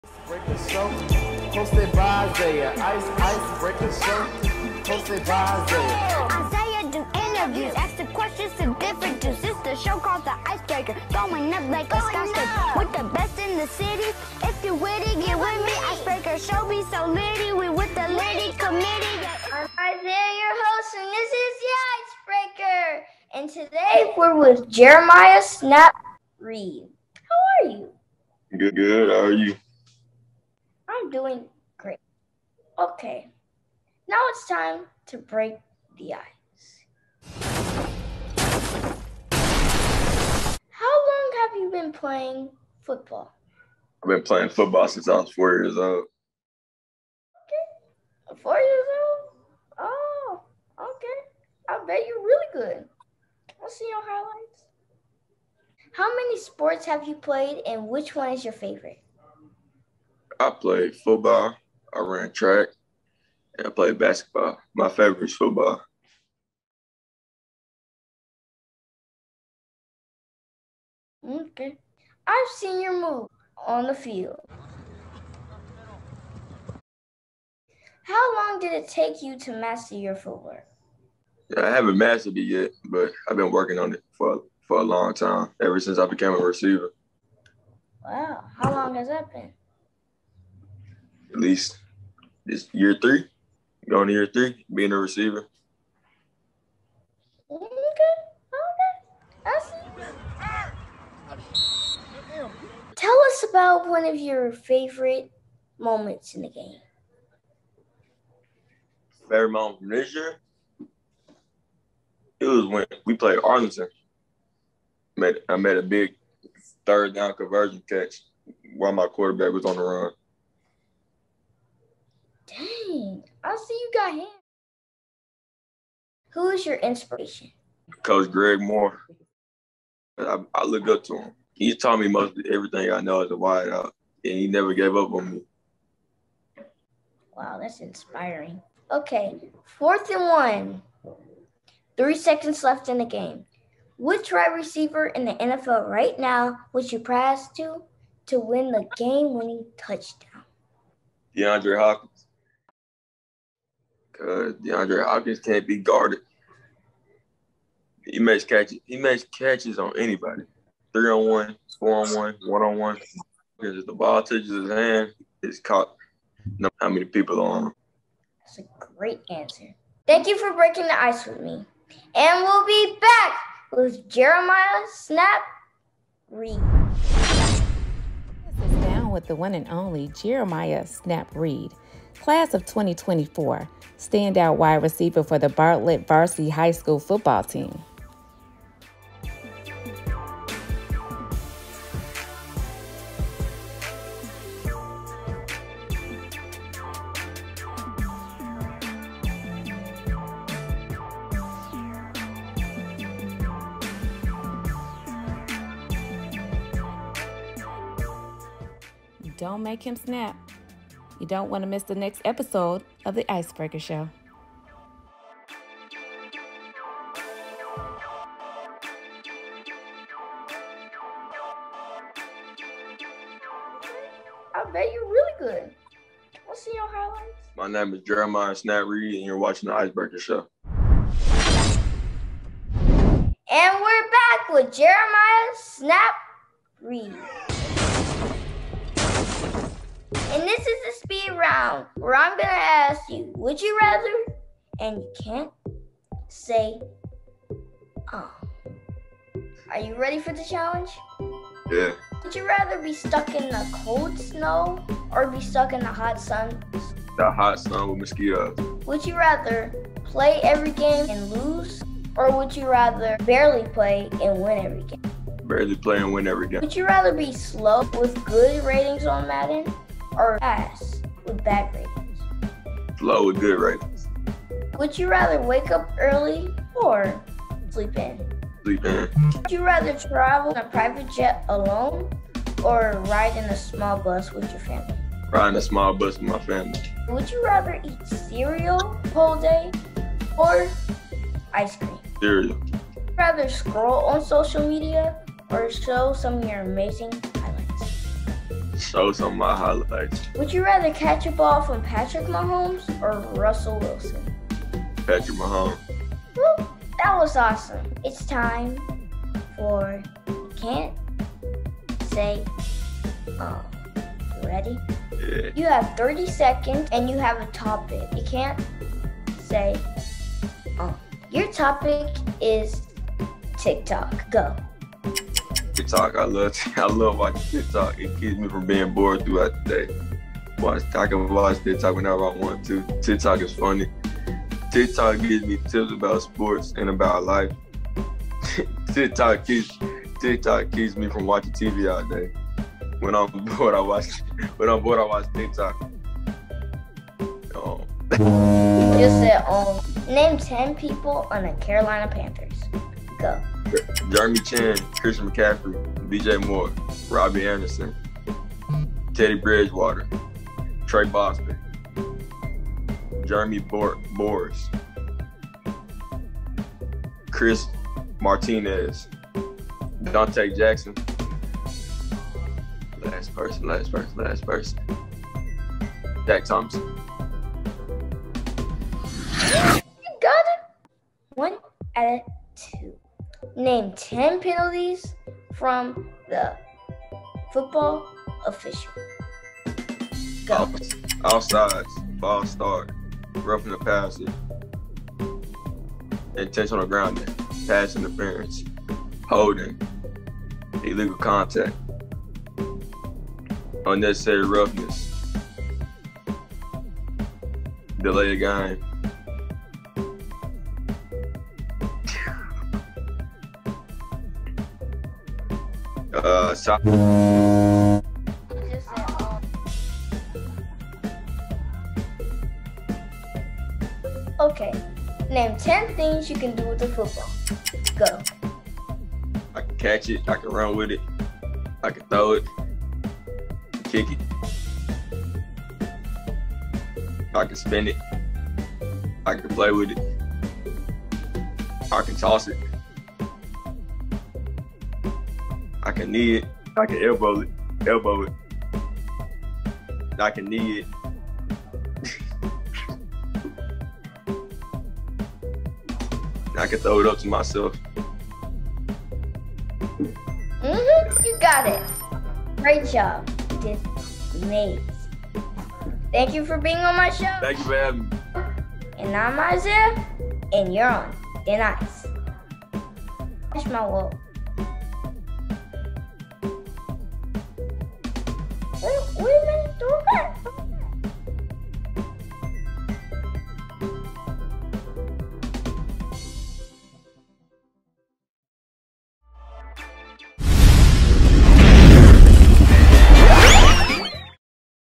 Ice Breaker Show - Season 3 - Episode 2 w/Jeremiah “Snap” Reed
HOST: Isaiah Legend aka Iceman

GUEST: Jeremiah “Snap” Reed, Class of 2024, WR, Bartlett Panthers Varsity Football Team